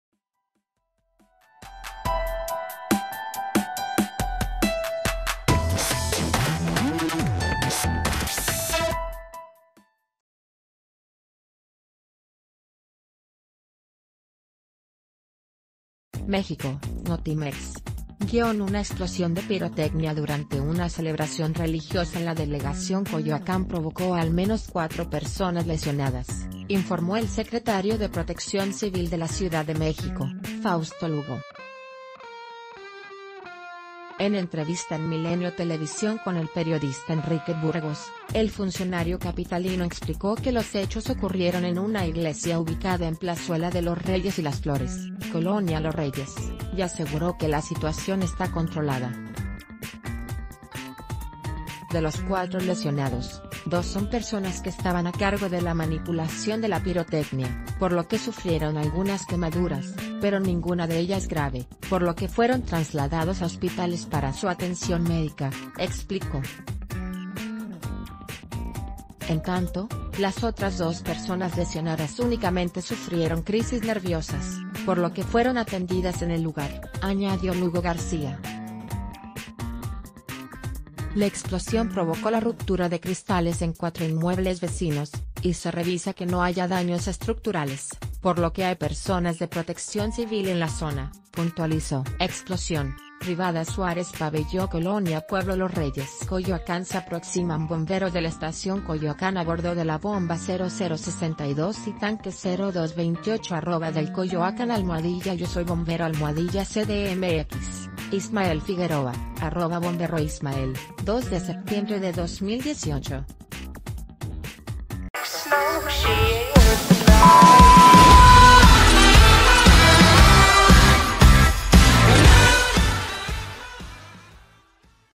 Se registra fuerte explosión de pirotecnia en Coyoacán - El Diario de Yucatán
México (Notimex).- Una explosión de pirotecnia durante una celebración religiosa en la delegación Coyoacán provocó al menos cuatro personas lesionadas, informó el secretario de Protección Civil de la Ciudad de México, Fausto Lugo. En entrevista en Milenio Televisión con el periodista Enrique Burgos,...